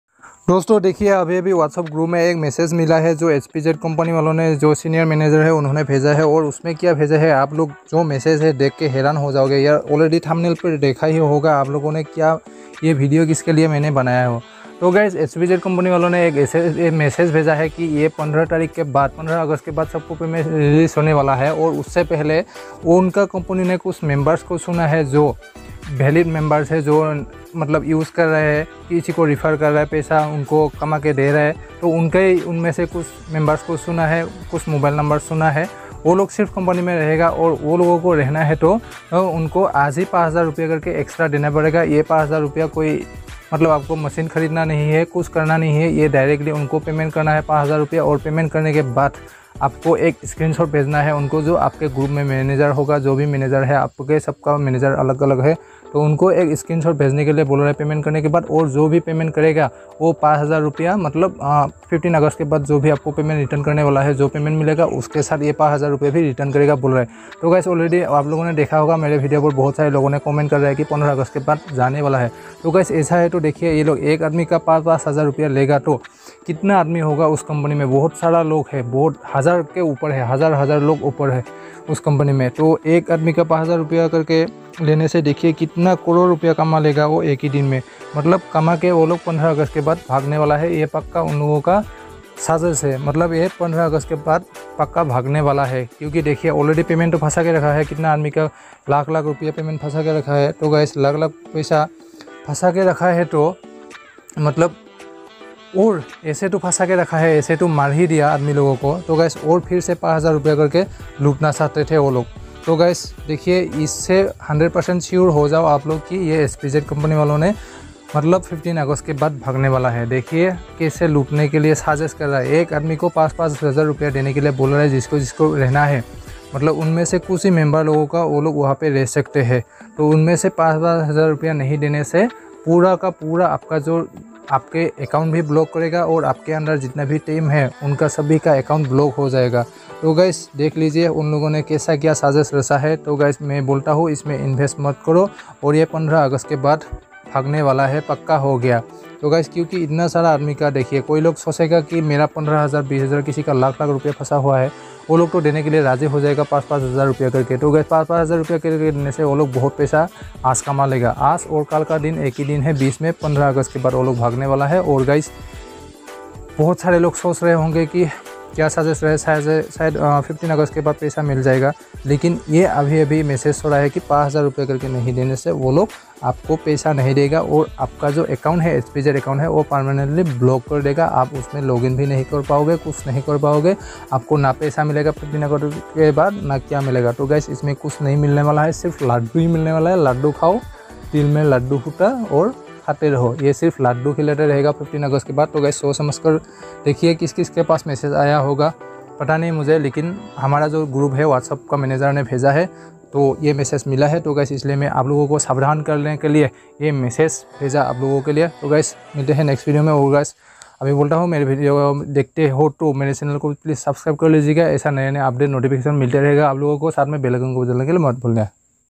Hpz company asking 5000rs if you not payment them your account will be banned now
अगर आपने 5 हजार रुपया नही दिया तो आपका Hpz Account बेंड हो जाये मतलब पक्का ये लोग भागने वाले है

📧Direct  massage to me Telegram